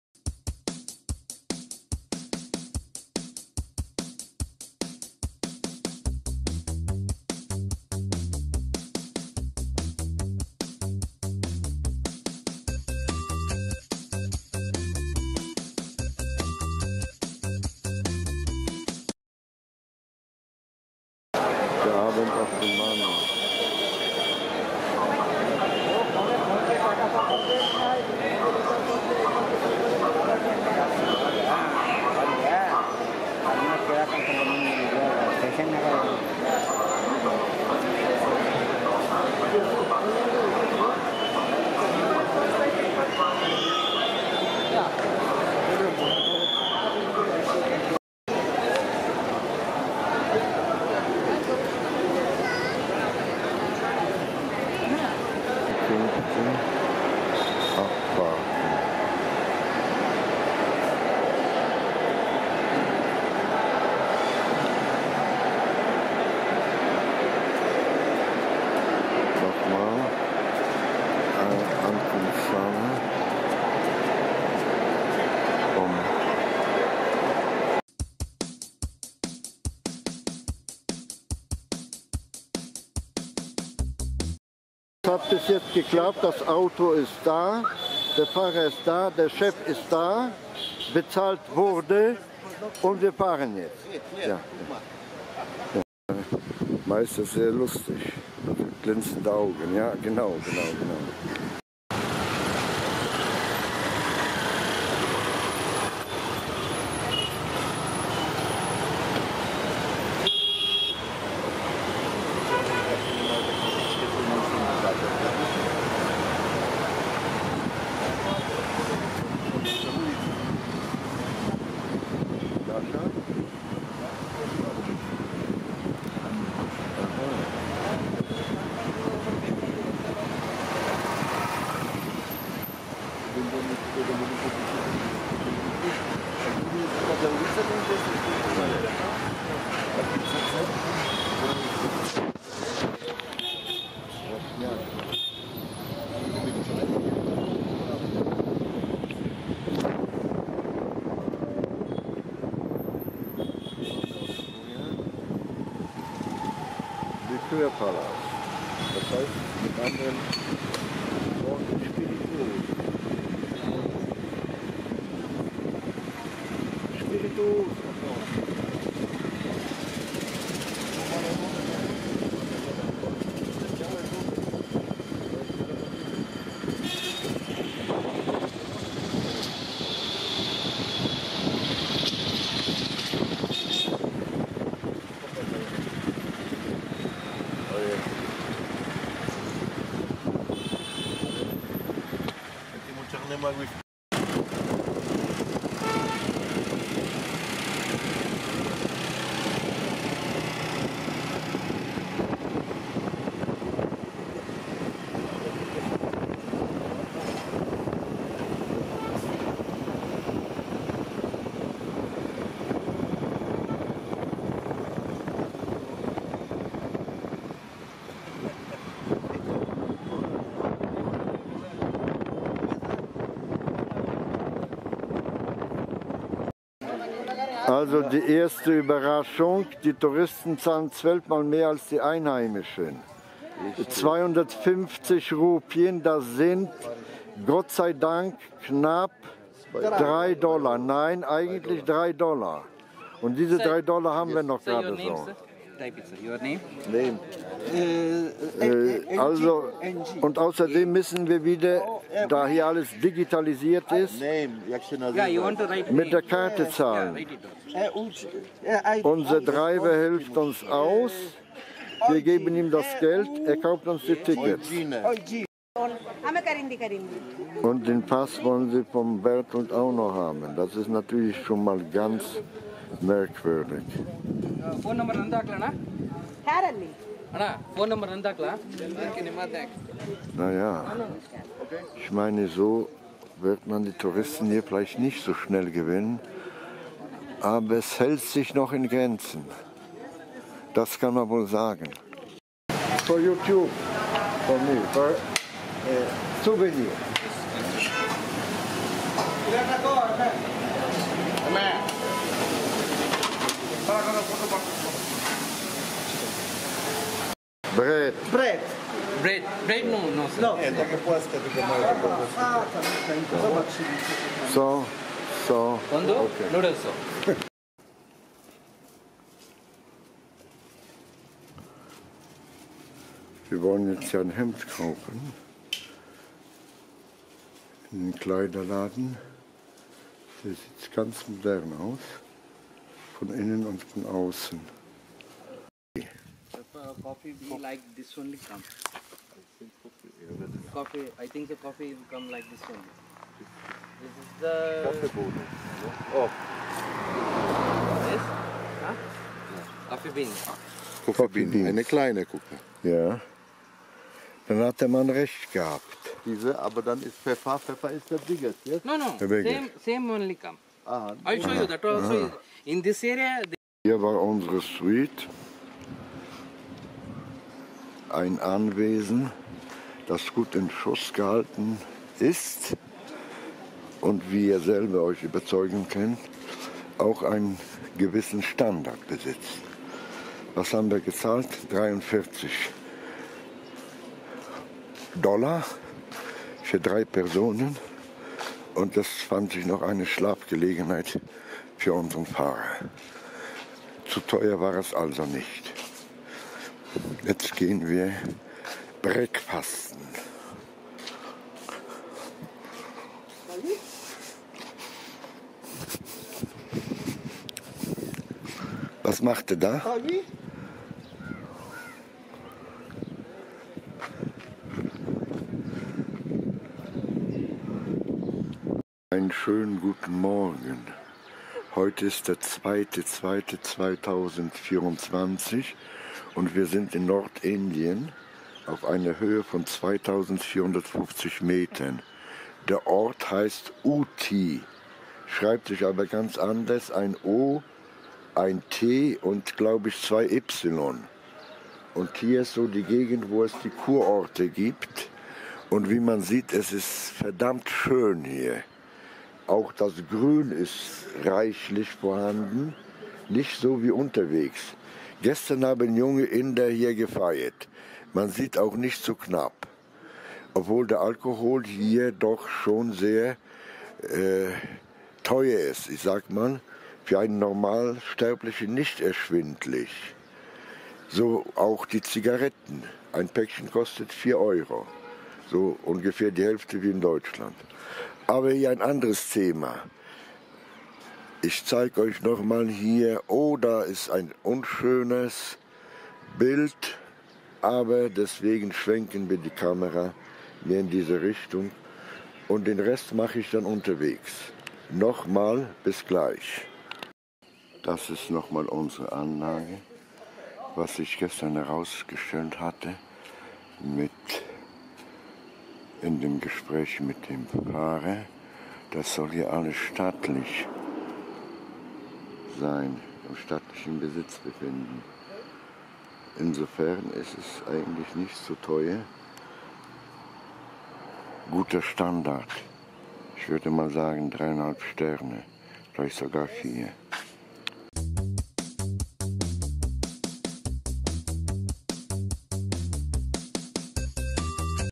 Indien - wie reist man im Lande als Tourist: Ohne Auto mit Fahrer keine Individualreise!
Von meiner Kurzreise nach Indien ein nächster YT-Bericht. Eine generelle Anmerkung: Kommt man sich z.B. in Lateinamerika mit Bussen überall hin, ist dies in Indien - obwohl theoretisch nicht verboten - ausgeschlossen. Keine Züge (ohne lange Voranmeldungen), keine Busse. So ist das!